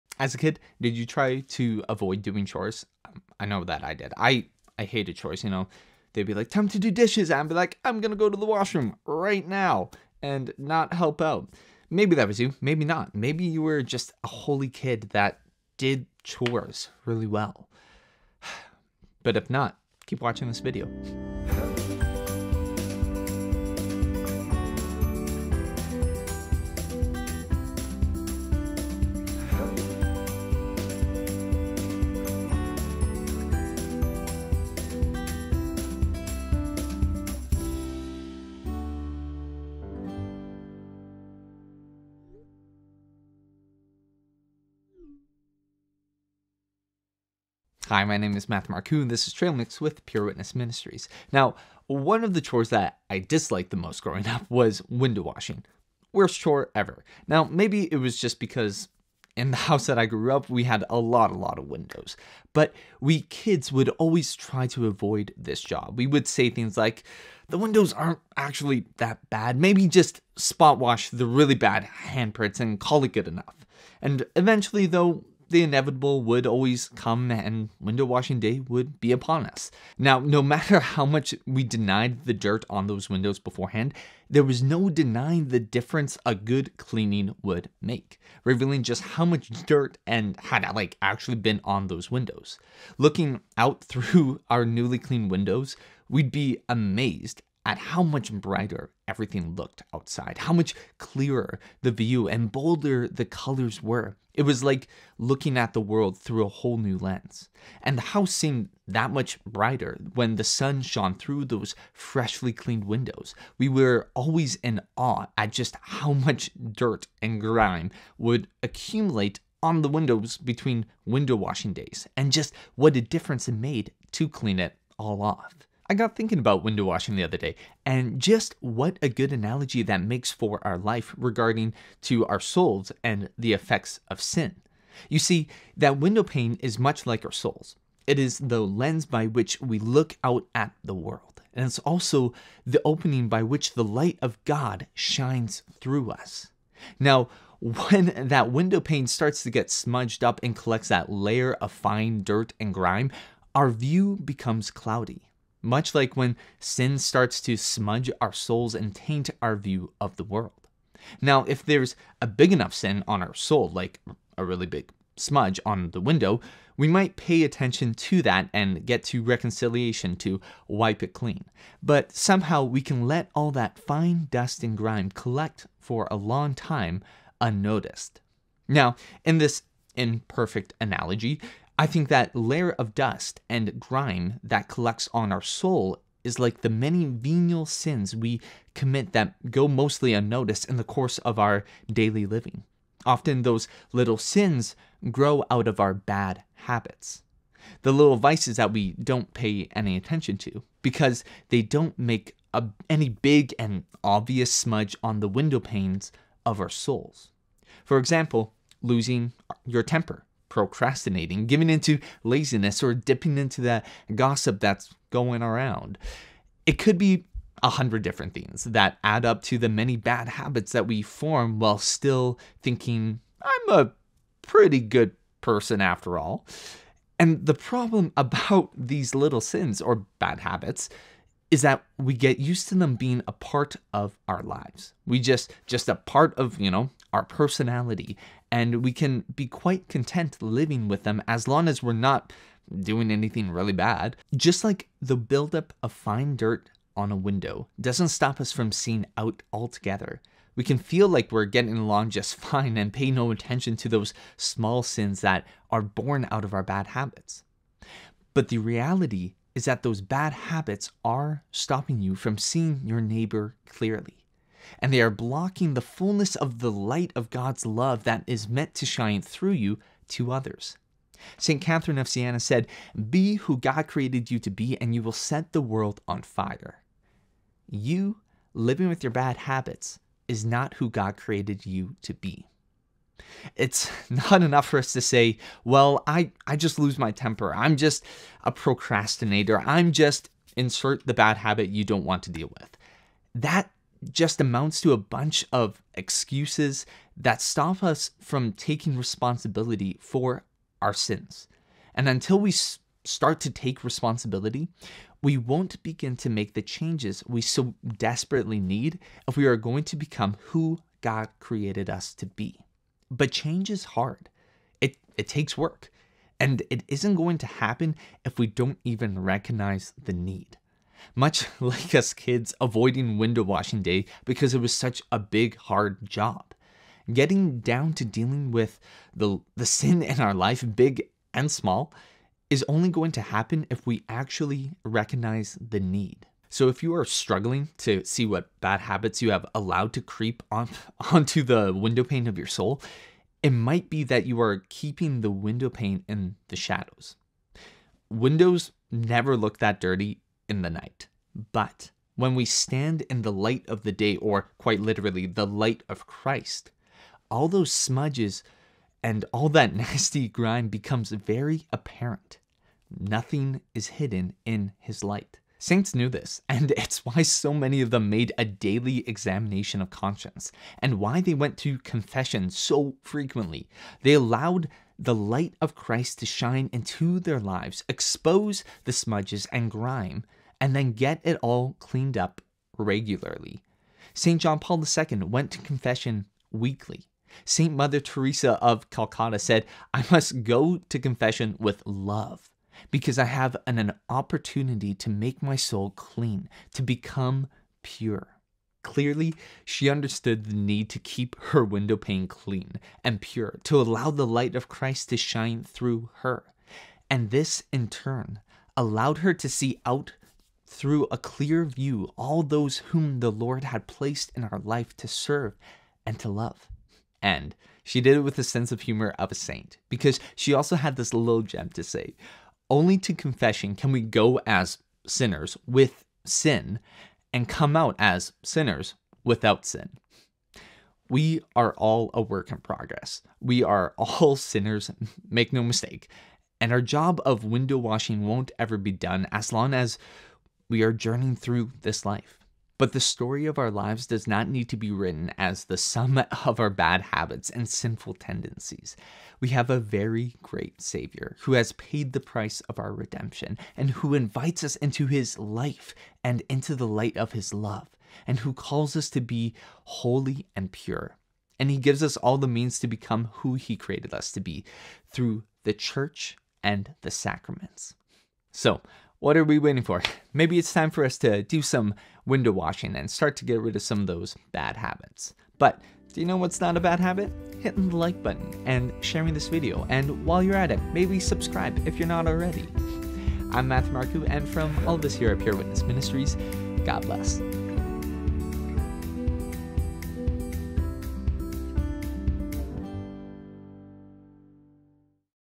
Bad Habits and Window Washing, Trail Mix with Matthew Marcoux
The story of our lives does not need to be written as the sum total of our bad habits and sinful tendencies.  We have a very great Savior who has paid the price of our Redemption, and who invites us into His life and into the light of His love and who calls us to be holy and pure.  In this week’s Trail Mix, Matthew Marcoux shares an analogy between window washing and the effects of bad habits in our life, with encouragement for us to clean up those bad habits, get to confession, and allow the light of Christ to shine brightly through us.